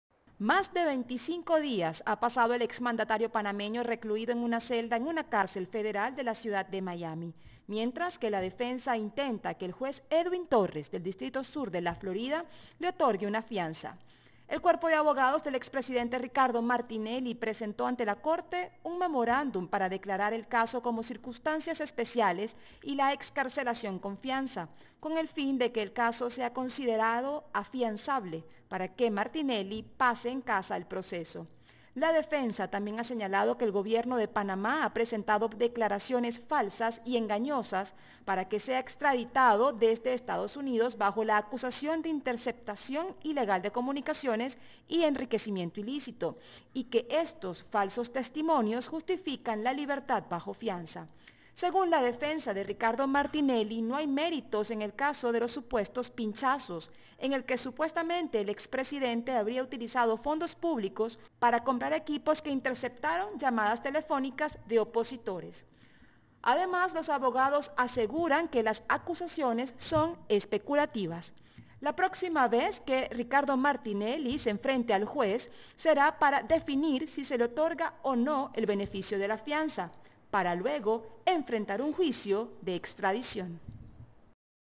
Expresidente Martinelli: “Panamá presentó declaraciones falsas a EE.UU.”
El expresidente panameño, Ricardo Martinelli, continúa detenido en Miami, en Estados Unidos, en donde espera un juicio que defina si procede o no su extradición para ser procesado por delitos de corrupción y violación de la privacidad. Entretanto, la defensa alega que el gobierno panameños ha mentido y presentado declaraciones falsas.
Más de 25 días ha pasado el exmandatario panameño recluido en una celda en una carcel federal de la ciudad de Miami, mientras que la defensa intenta que el juez Edwin Torres del Distrito Sur de la Florida le otorgue una fianza a Martinelli.

El cuerpo de abogados del expresidente presentó ante la corte un memorándum para declarar el caso como circunstancias especiales y la excarcelación con fianza, con el fin de que el caso sea considerado afianzable para que su cliente Martinelli pase en casa el proceso.

La defensa también ha señalado que el gobierno de Panamá ha presentado declaraciones falsas" y "engañosas" para que sea extraditado desde Estados Unidos bajo la acusación de interceptación ilegal de comunicaciones y enriquecimiento ilícito, y que estos falsos testimonios justifican la libertad bajo fianza.

Según la defensa, no hay méritos en el caso de los supuestos pinchazos, en el que supuestamente el expresidente habría utilizado fondos públicos para comprar equipos que intercepataron las llamadas telefónicas de opositores Además los abogados aseguran que las acusaciones son especulativas.

La próxima audiencia que enfrentará Martinelli será para definir si se le otorga o no el beneficio de la fianza, para luego enfrentar un juicio para su extradición.